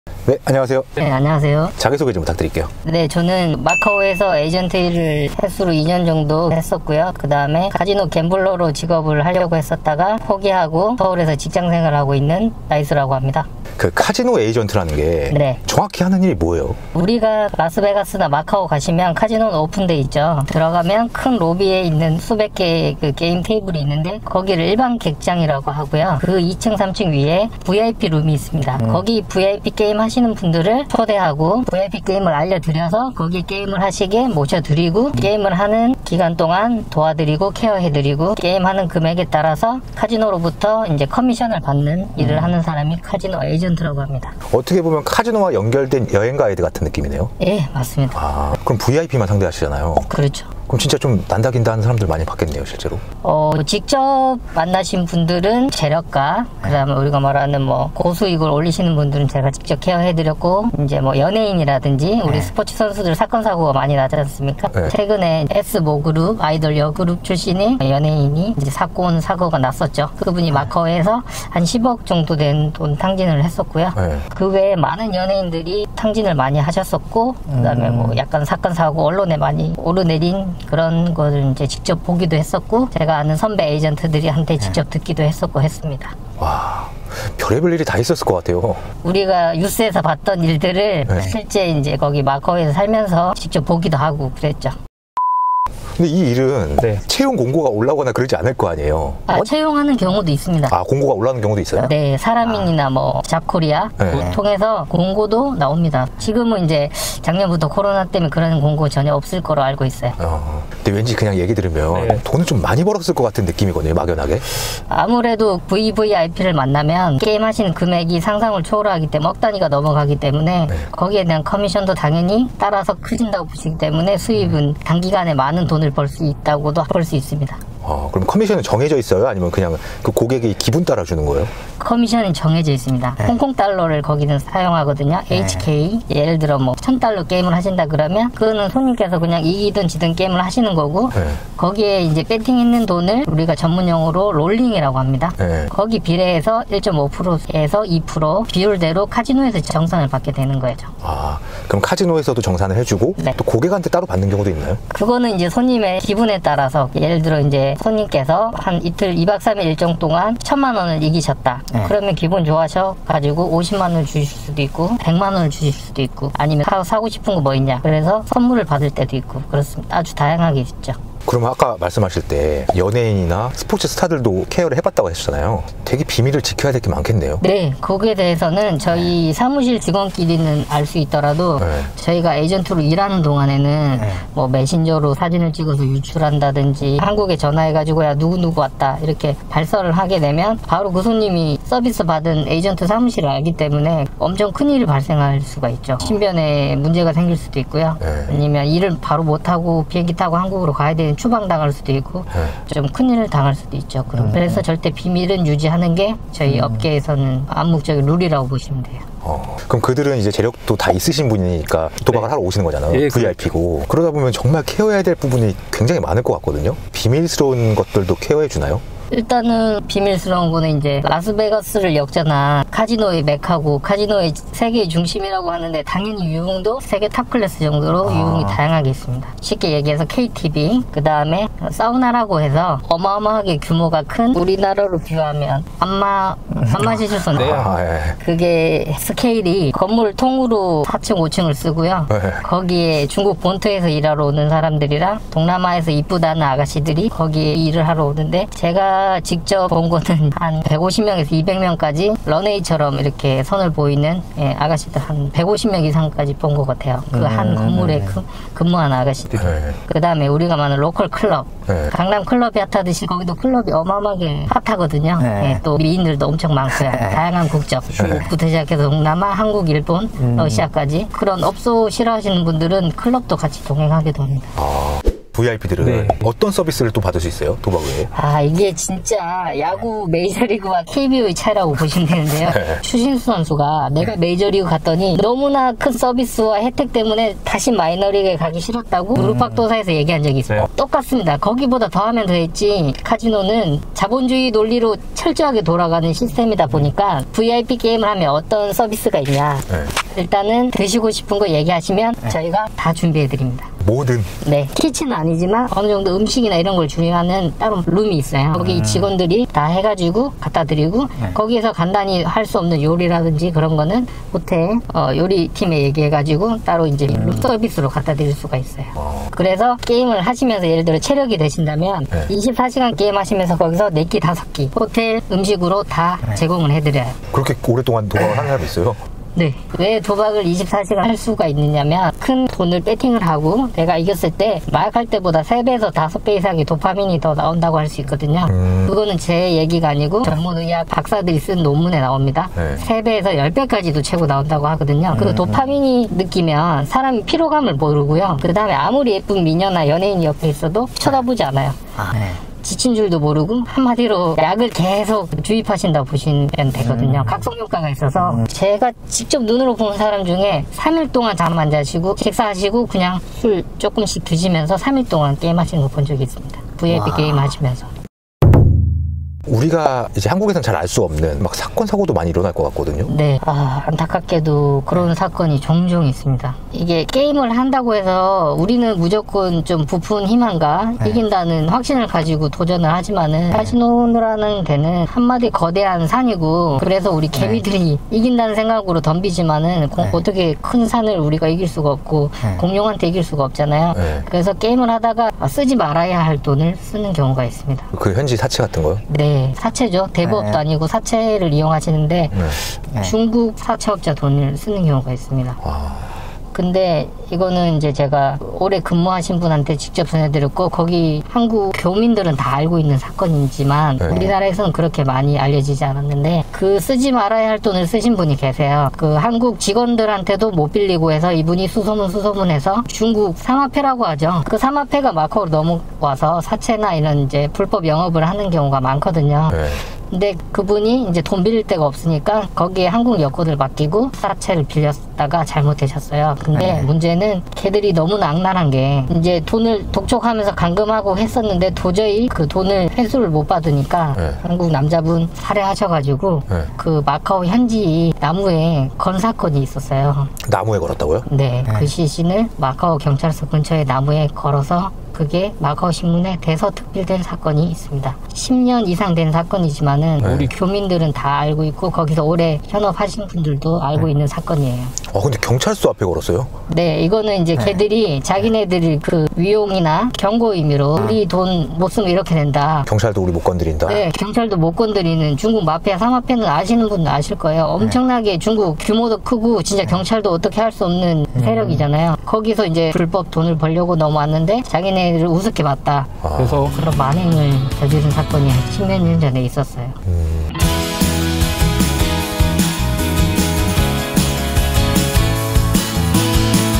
해외 도박 하다 중국 조폭에게 돈 빌리면 생기는 충격적인 일 | 마카오 카지노 에이전트
안녕하세요
오늘은 마카오 카지노 에이전트에서 일을 한 나이스님의
이야기를 들어보았습니다.